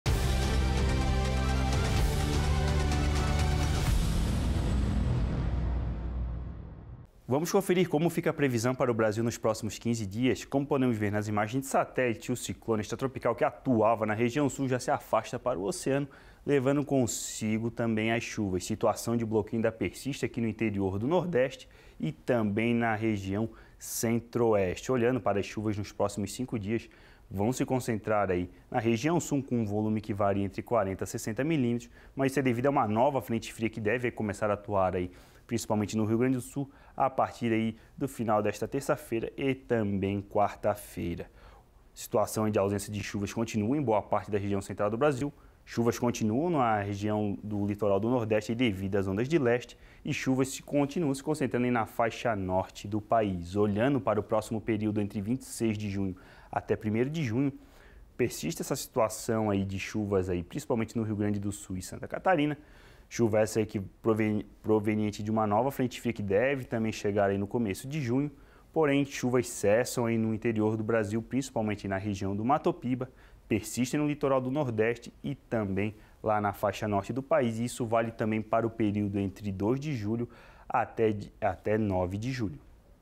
Previsão do tempo: Brasil 15 dias | Chuvas vão persistir no sul e litoral do nordeste | 19/6/23
Temperaturas devem subir ao longo da semana